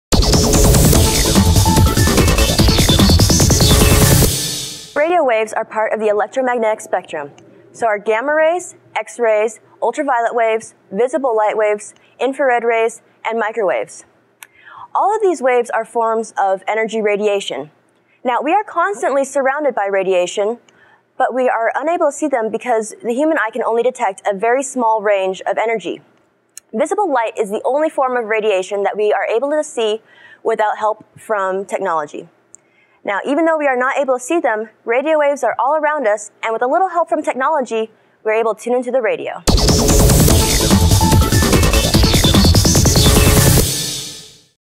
Why Can't We See Radio Waves?
Arizona Science Center answers, "Why Can't We See Radio Waves?".
Never Stop Wondering.